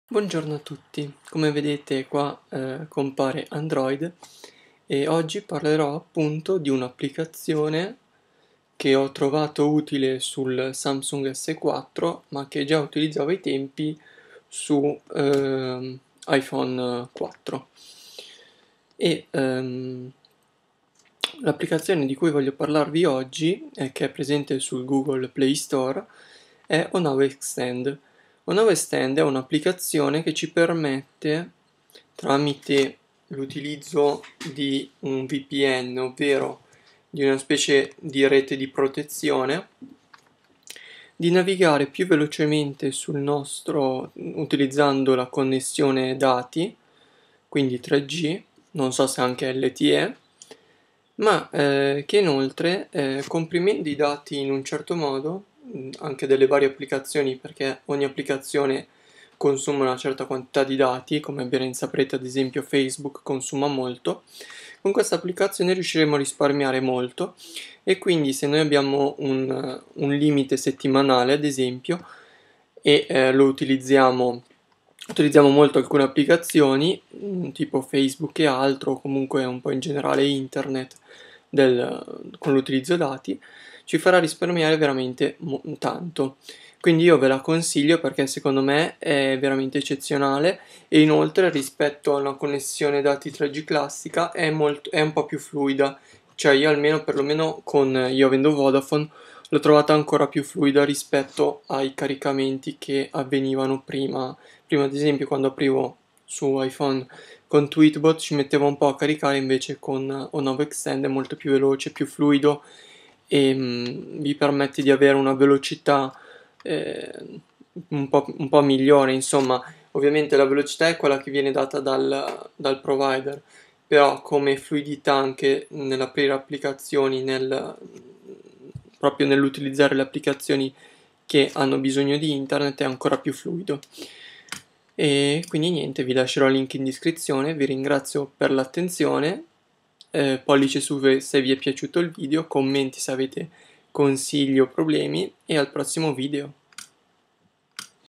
Onavo Extend per Android!
If you want to see a specific video, just ask in comments down below :) Recensione dell'applicazione Onavo Extend che ci permette di risparmiare l'uso di dati 3g comprimendo i dati usati dalle app.